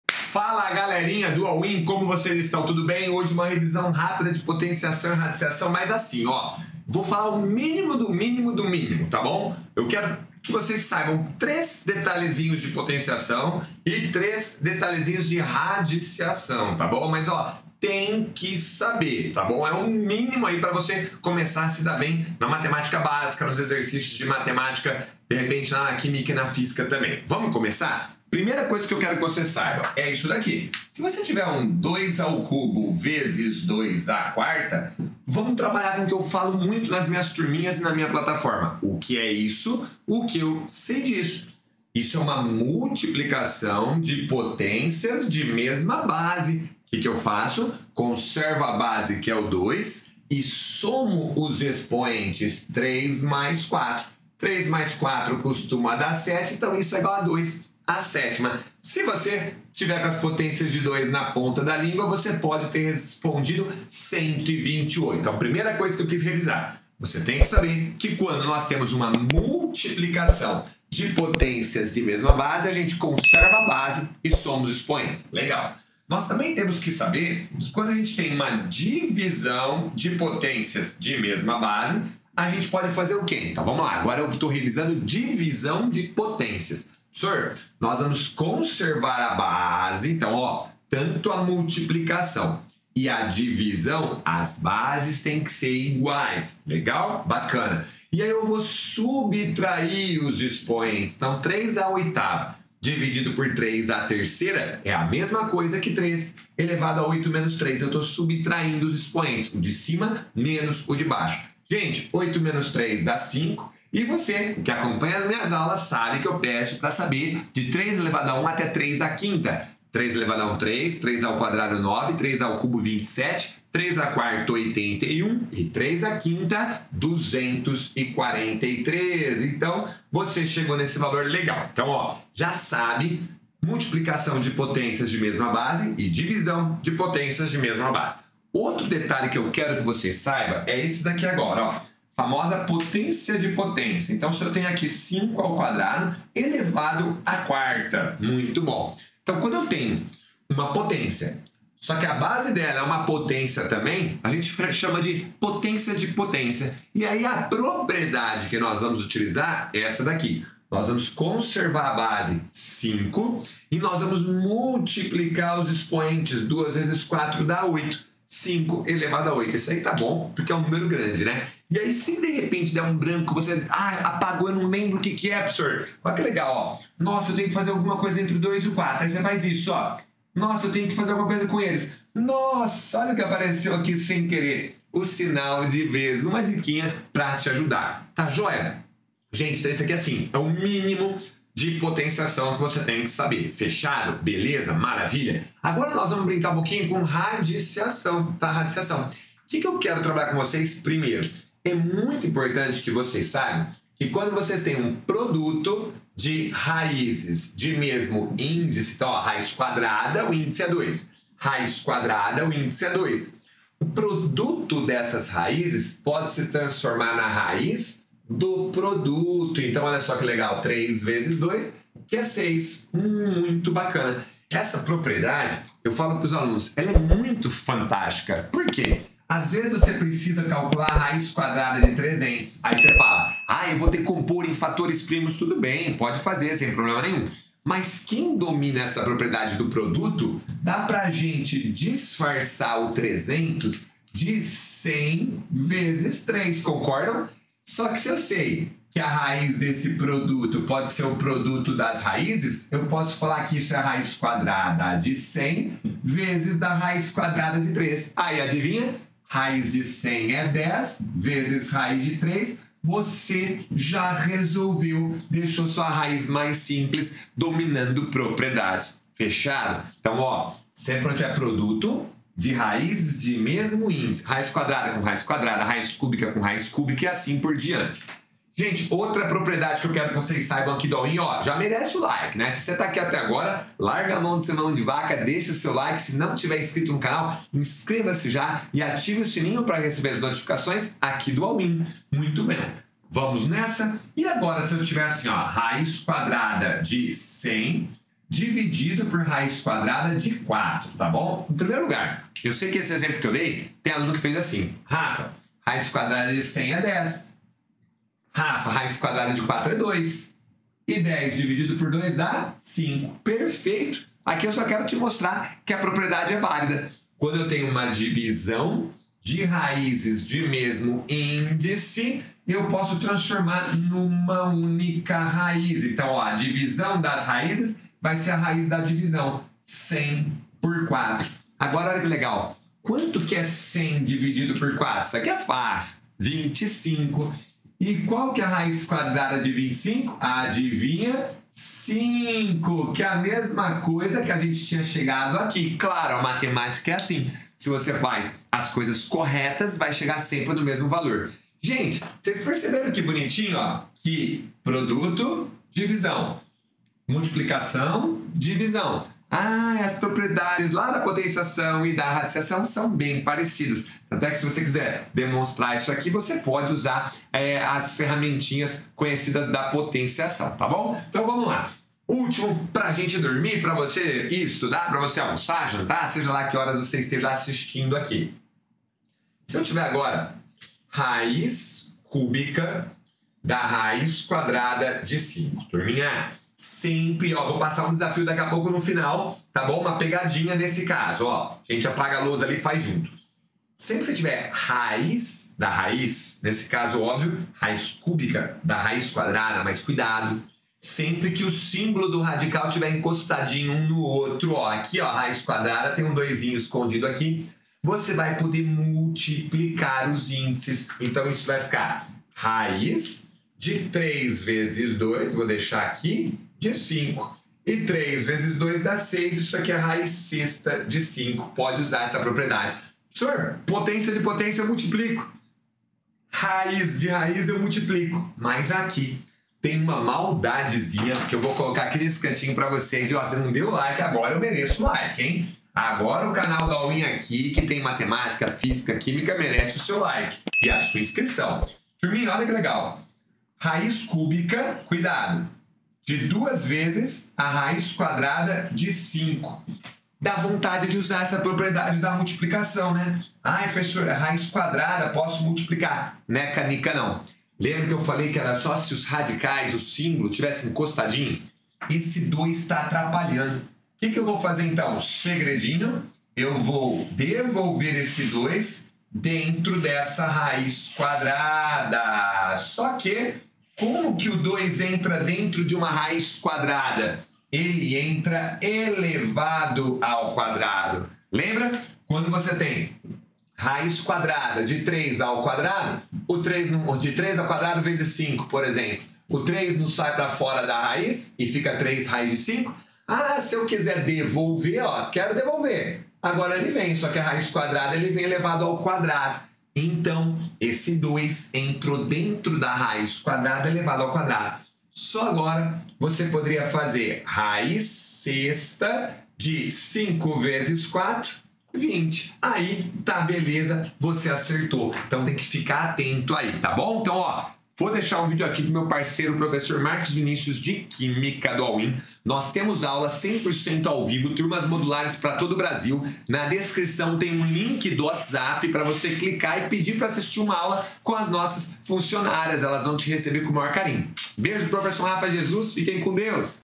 Você tem que saber!!!! Revisão de Potenciação e Radiciação!
A potenciação ou exponenciação é a operação matemática que representa a multiplicação de fatores iguais. Ou seja, usamos a potenciação quando um número é multiplicado por ele mesmo várias vezes.
Radiciação é a operação que realizamos quando queremos descobrir qual o número que multiplicado por ele mesmo determinada quantidade de vezes, dá um valor que conhecemos.

Fala galera! mais uma aula do professor Rafa Jesus. Bons estudos!

Só aqui no All Win cursos.

ALL WIN CURSOS